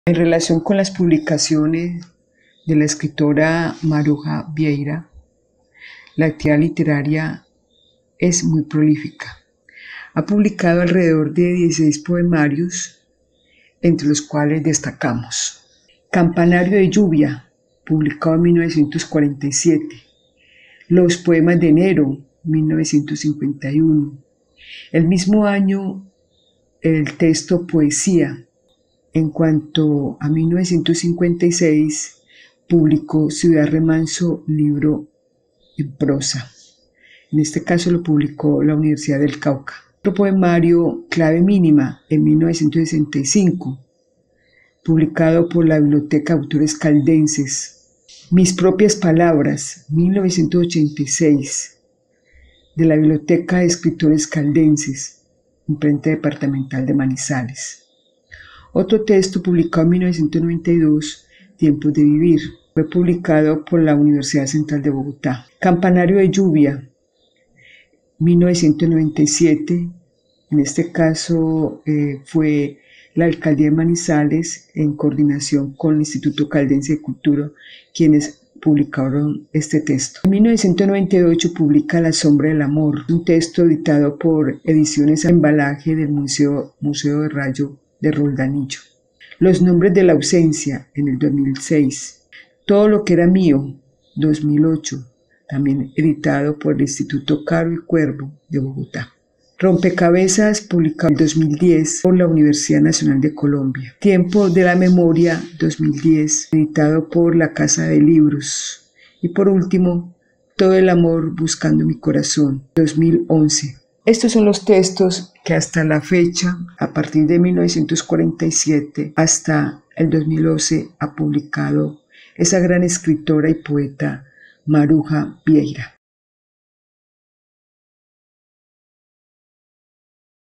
Publicaciones de Maruja Vieira
Les compartimos las diferentes publicaciones de la escritora Maruja Vieira en la voz de nuestra musa coordinadora Magaly Pacheco Marimón.